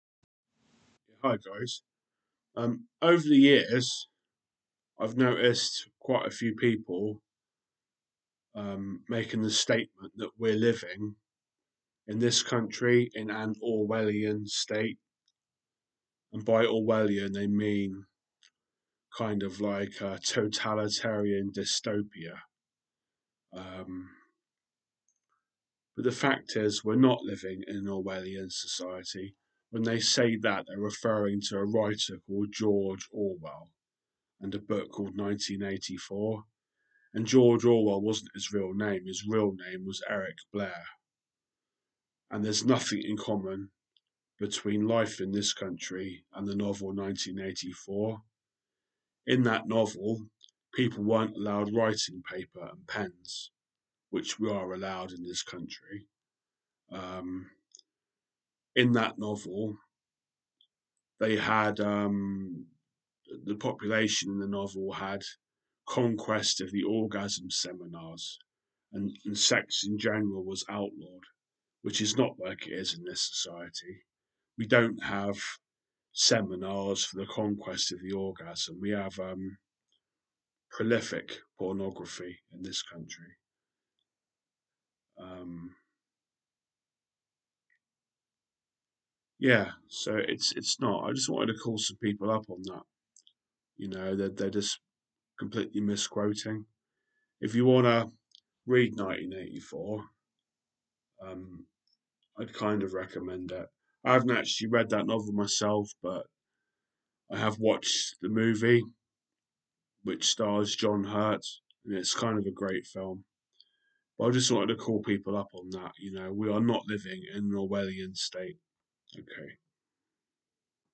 Orwellian State?
Just calling out people who misquote Eric Blair a.k.a. George Orwell; the man who wrote the famous novel '1984'.